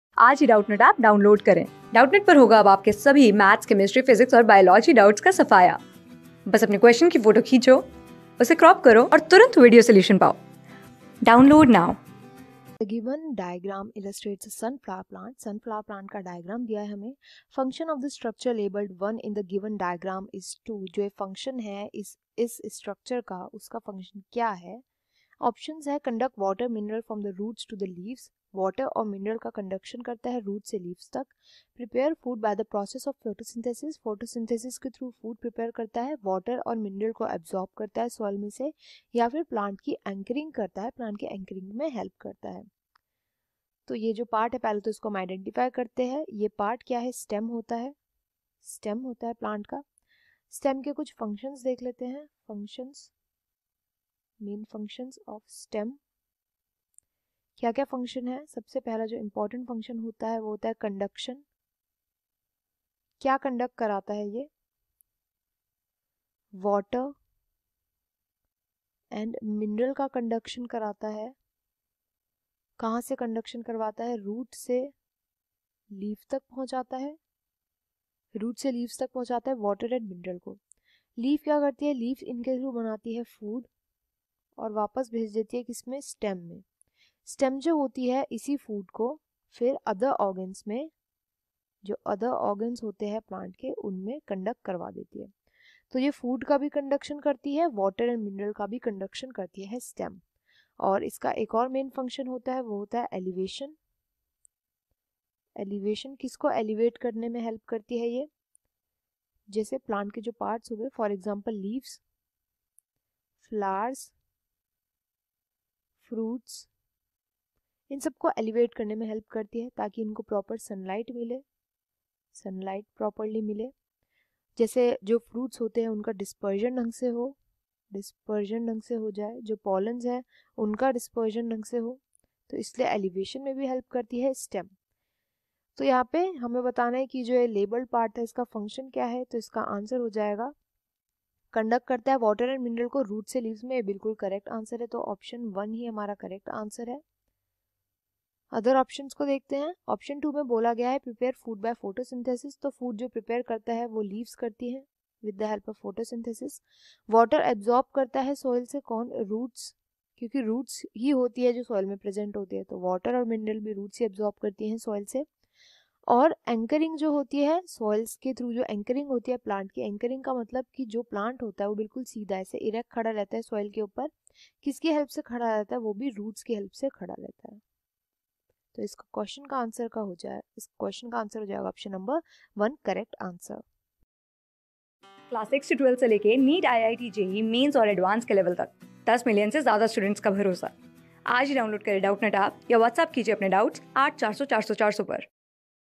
The given diagram illustrates a sunflower plant. The function of the structure labelled I in the...
The given diagram illustrates a sunflower plant. The function of the structure labelled I in the given diagram is to
Class: 6
Subject: BIOLOGY
Chapter: STRUCTURE AND FUNCTIONS OF PLANT PARTS
Board:FOUNDATION

👉 Have Any Query? Ask Us.

Our Telegram Pages:
class 6
6 class ka math syllabus